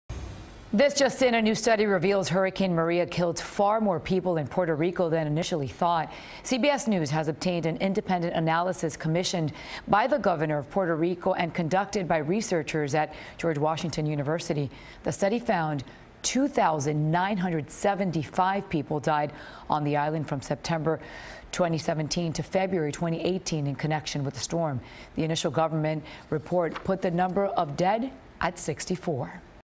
Hurricane Maria Killed More People In Puerto Rico Than Originally Thought
Study estimated nearly three thousand people were killed, the initial government death toll was 64.